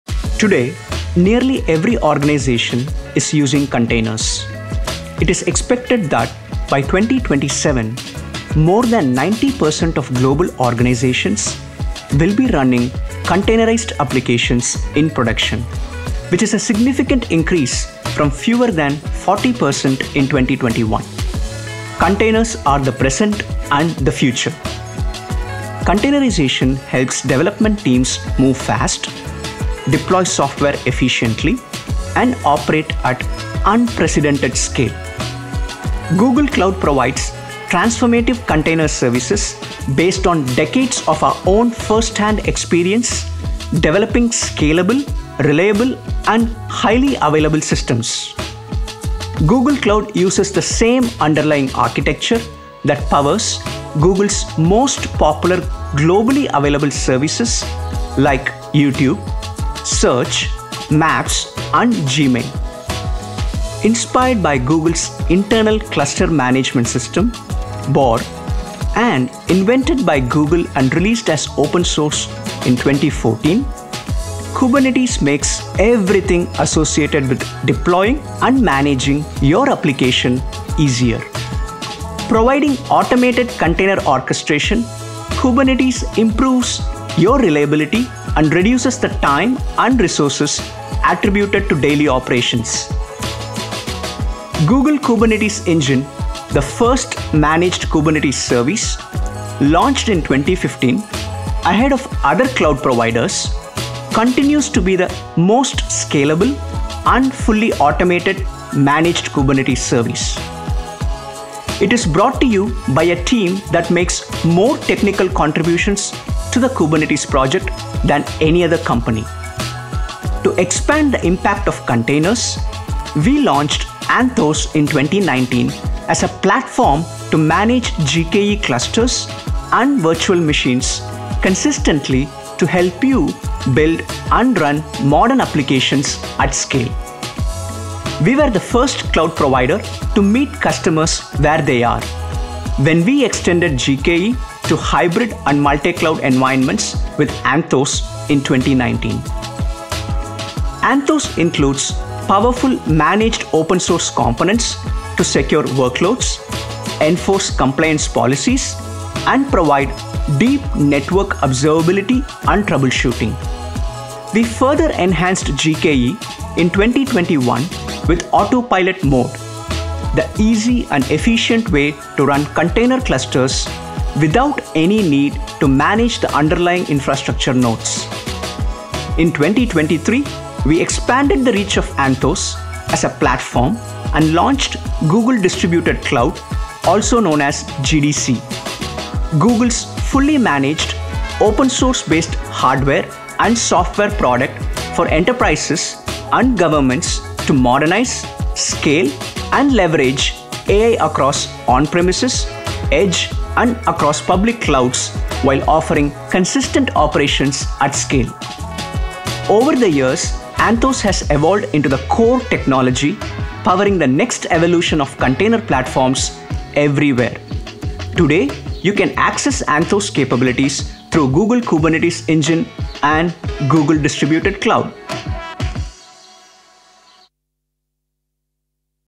Anthos evolution
What is Anthos? How has it evolved? Anthos was launched in 2019 as a platform to manage Google Kubernetes Engine (GKE) clusters and virtual machines consistently and to help you build and run modern applications at scale. Anthos includes powerful managed open source components to secure workloads, enforce compliance policies, and provide deep network observability and troubleshooting. Over the years, Anthos has evolved into the core technology powering the next evolution of container platforms everywhere. Today, you can access Anthos capabilities through Google Kubernetes Engine (GKE) and Google Distributed Cloud (GDC).